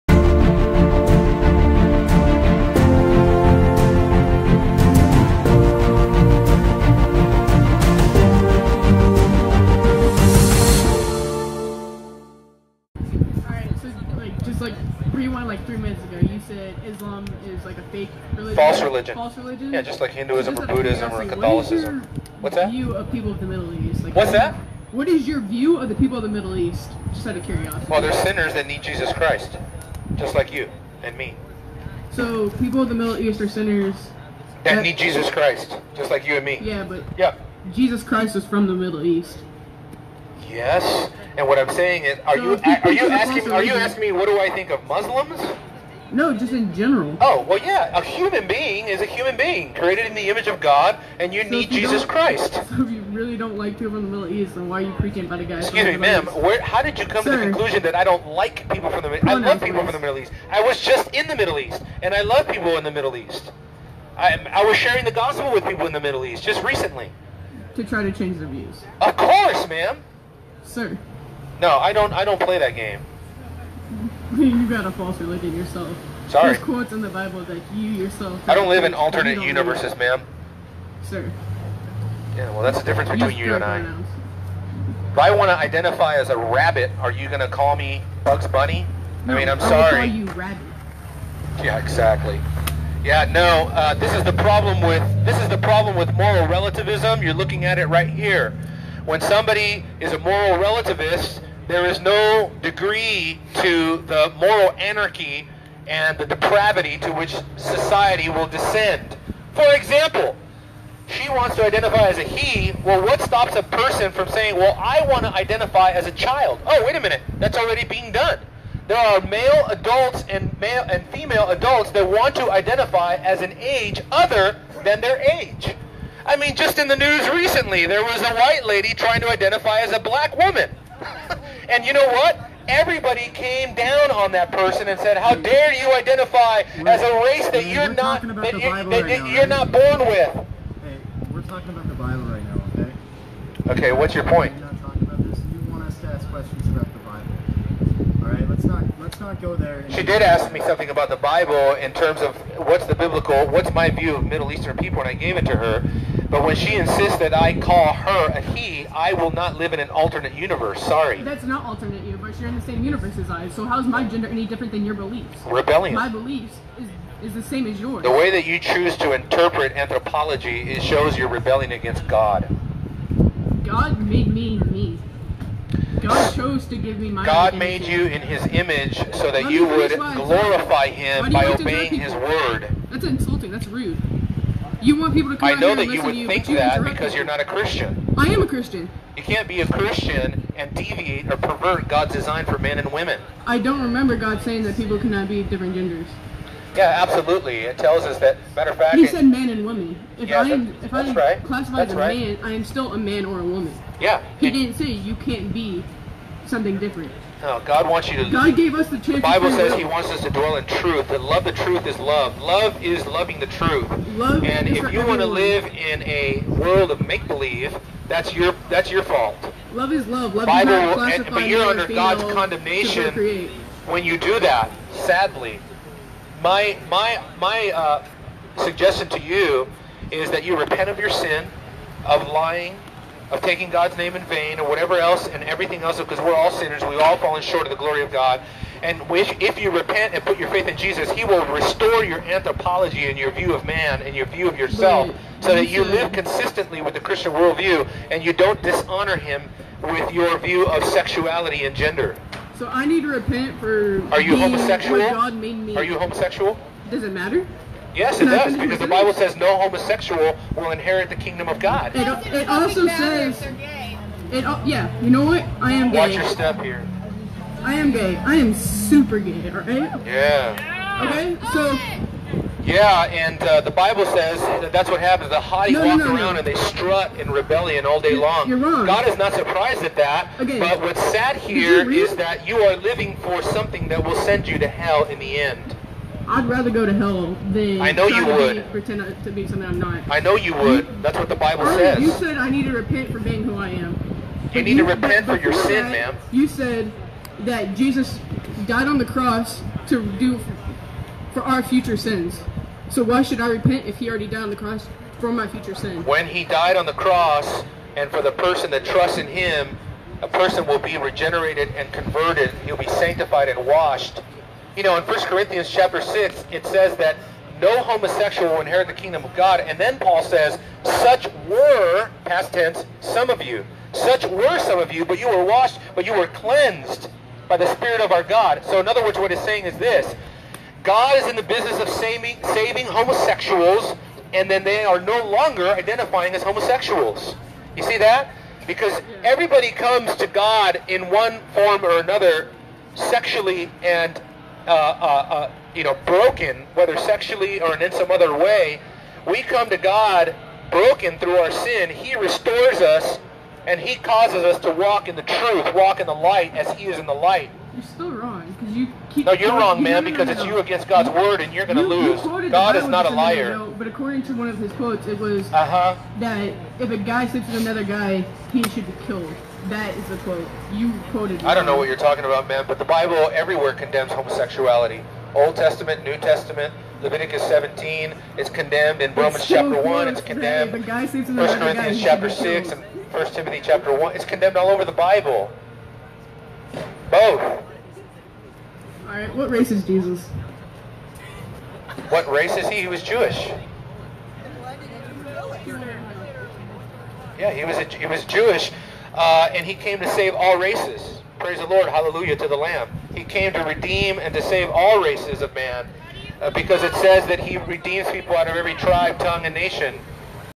Christian Pastor Encounters Transgender Delusion | University Of North Texas Preaching
Pastor Emilio Ramos out again at the University of North Texas engaging with a female student that identifies as transgender.

Every week we get to capture the delusion these students live in with a post modern mindset and worldview.

For a great resource on apologetics and evangelism